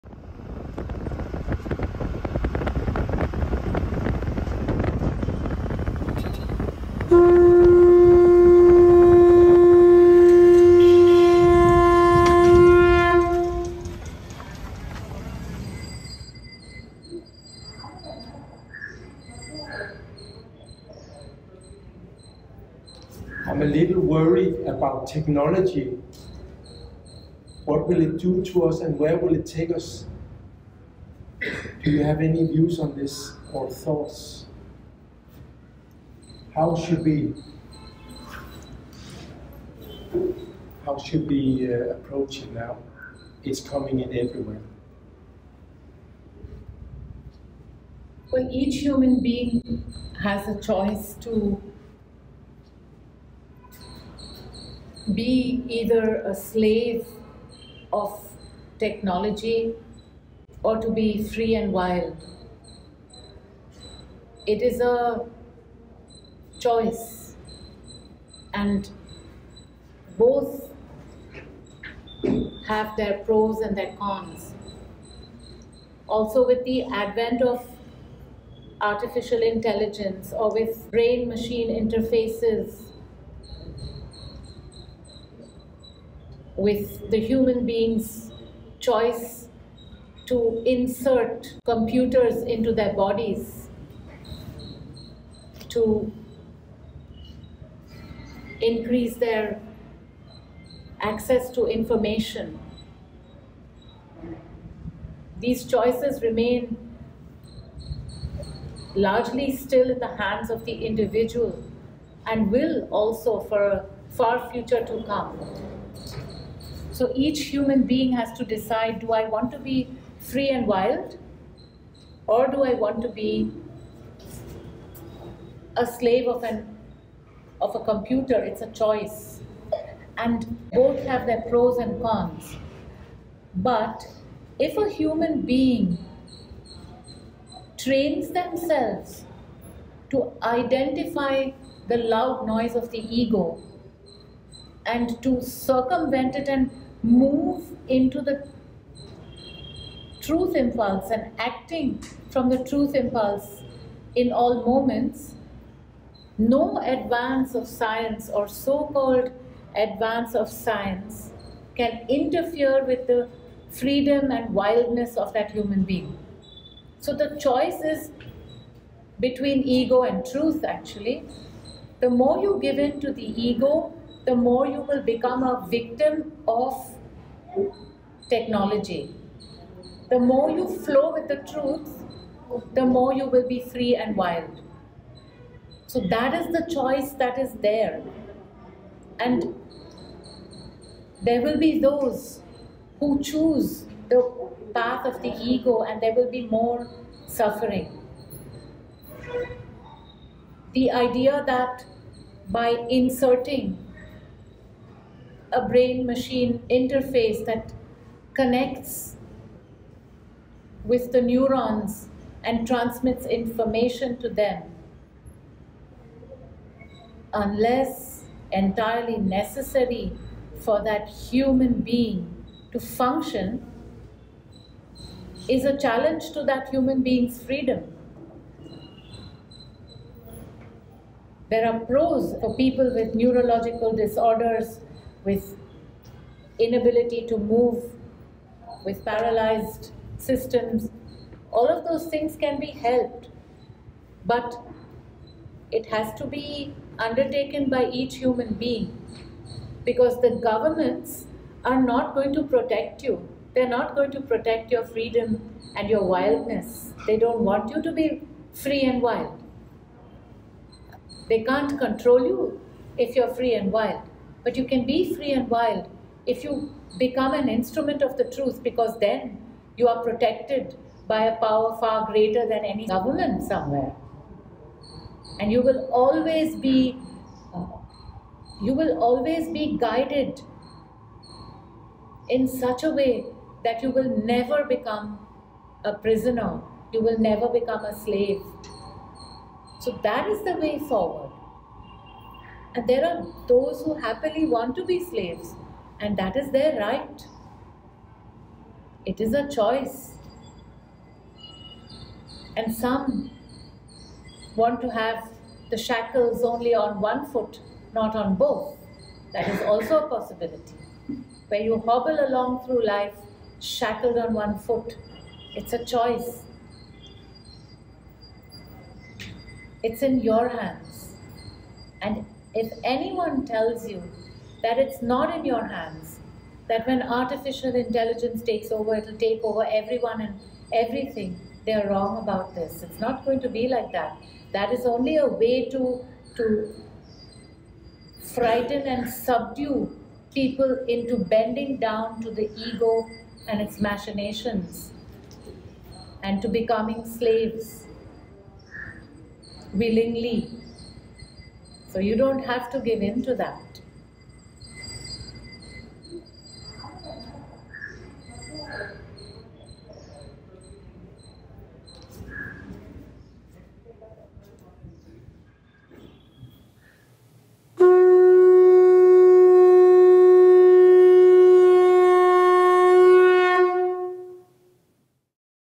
Maharishikaa | AI - Tech slave or wild and free?
A worried Dermot asks the Maharishikaa where we are headed with technology, and how it should be approached.

In this revelatory answer the Maharishikaa speaks about AI, brain interface technologies and how one can practically avoid the negative impact of technology by learning to act from the Truth Impulse - how each one has a choice - to be either a slave of technology or to be wild and free.